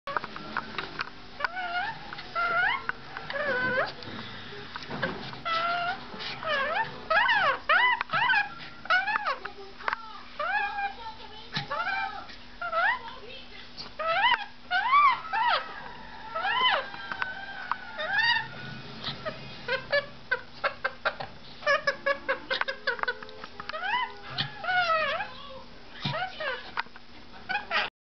Inky the Rainbow Lorikeet testing Samson's patience
You've heard of 'Charlie Bit My Finger' ... this is 'Inky Bit My Moustache'